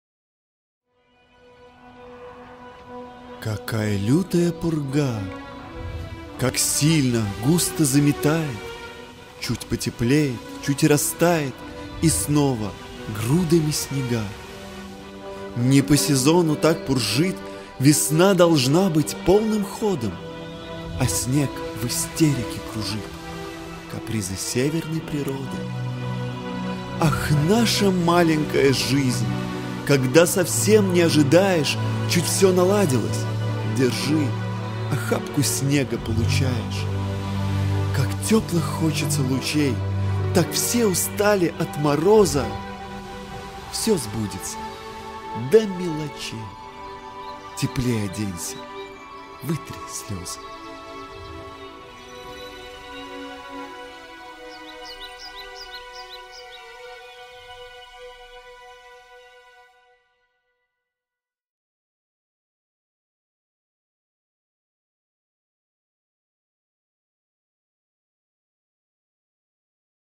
Какая лютая пурга - Видеопоэзия - Лансере
Стихотворение Наталии Лансере, читает Егор Лансере.
Какая лютая пурга
Какая лютая пурга!
Как сильно, густо заметает!
Чуть потеплеет, чуть растает,
И снова - грудами снега…

Не по сезону так пуржит,
Весна должна быть полным ходом;
А снег в истерике кружит…
Капризы северной природы!

Ах, наша маленькая жизнь...
Когда совсем не ожидаешь,
Чуть всё наладилось - держи,
Охапку снега получаешь.

Как теплых хочется лучей,
Так все устали от мороза!..
Всё сбудется, до мелочей.
Теплей оденься, вытри слёзы.